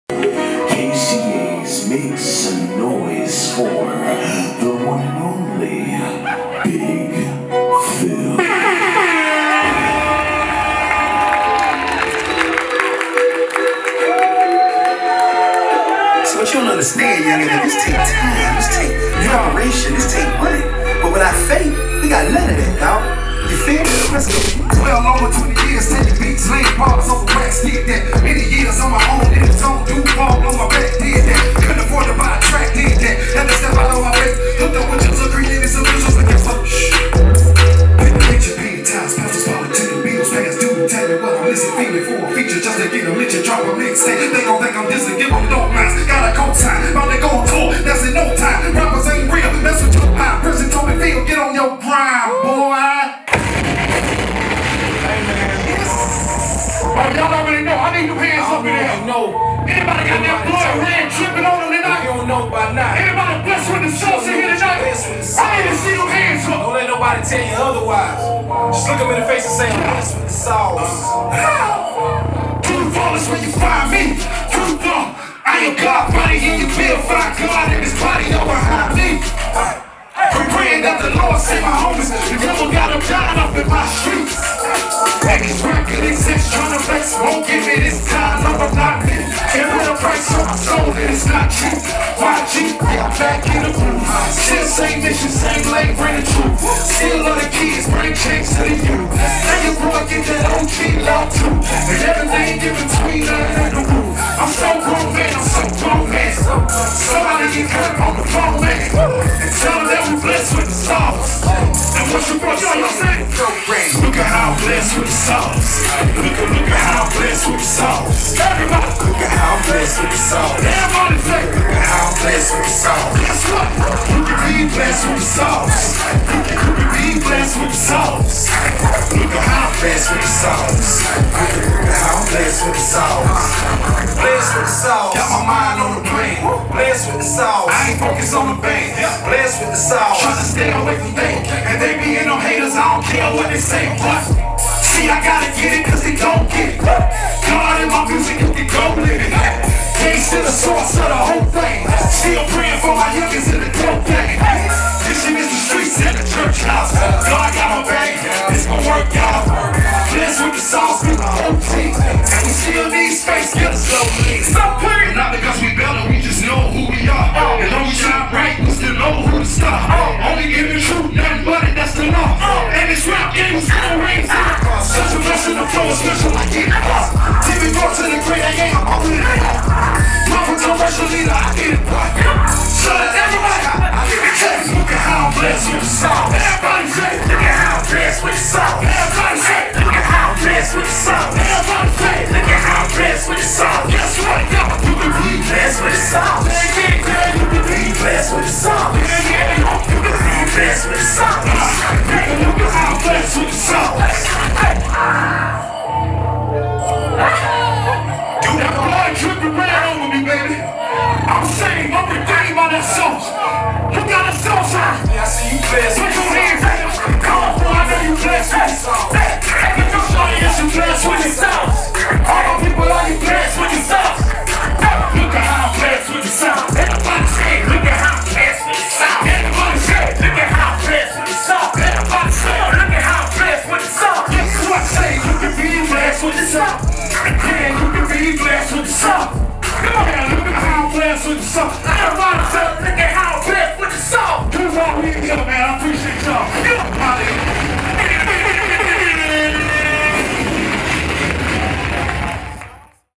Big Fil - Kingdom Choice Awards 2019 (live performance video)
Jacksonville heavy spitter, Big Fil delivered a dope performance live from the 9th Annual Kingdom Choice Awards in Brooklyn, NY. Duval was in the building heavy!